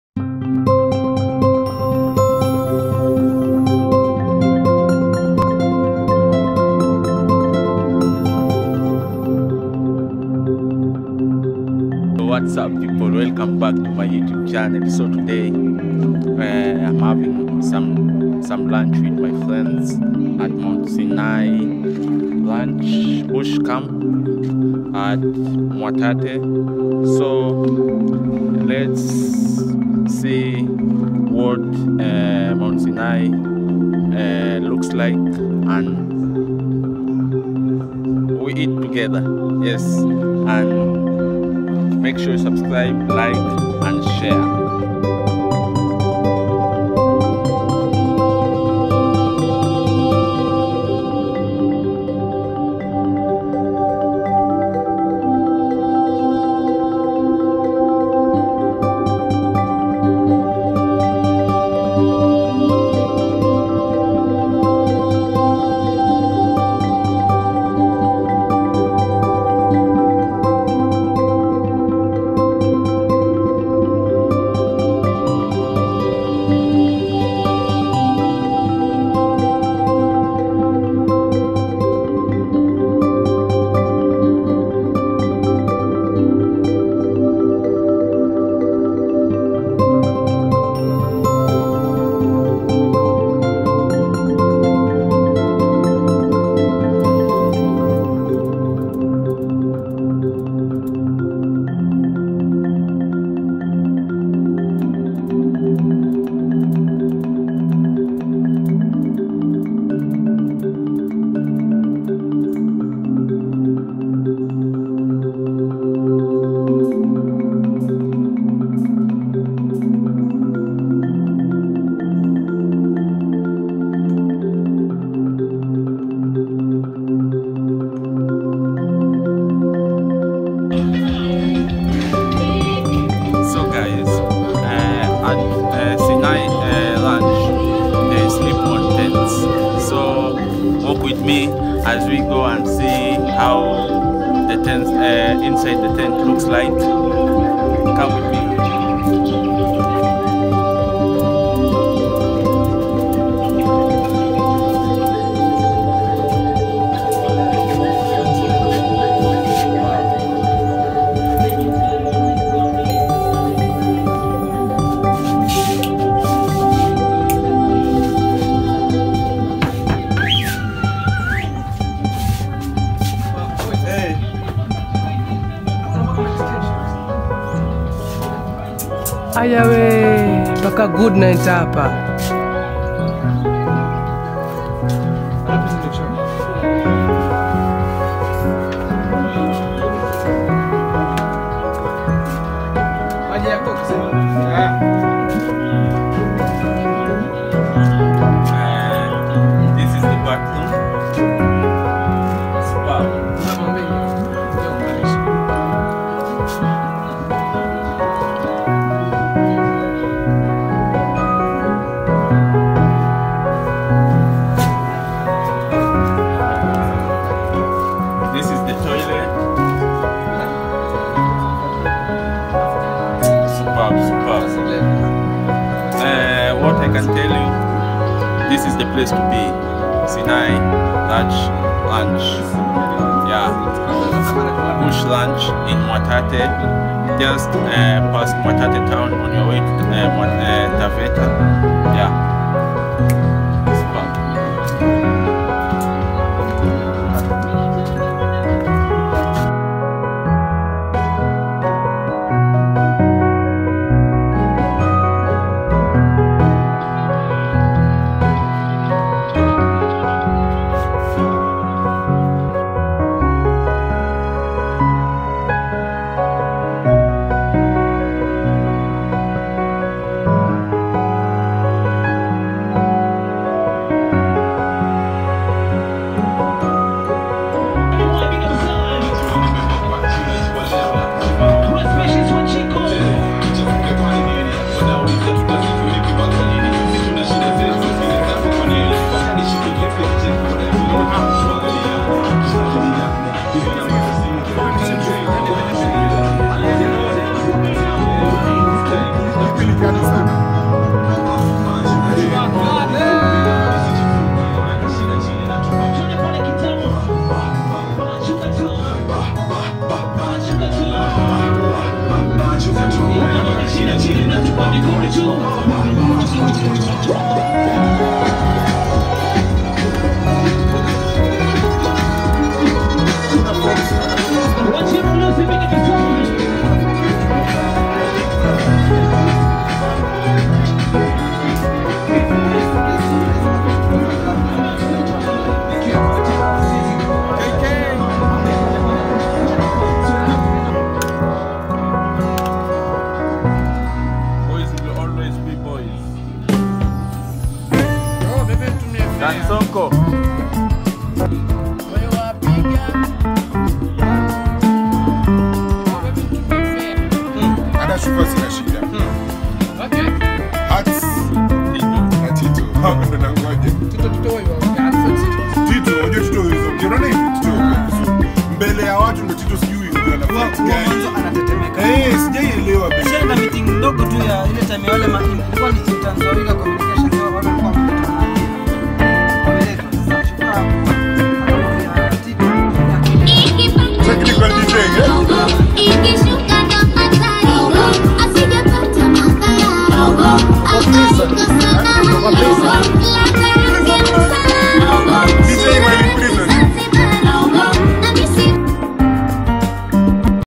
ADVENTURE IN THE BUSH - SINAI RANCH BUSH CAMP MWATATE VOI
Lock down was lifted and it was time to adventure into the bush and see what the sinai ranch bush camp had to offer. We were not disappointed.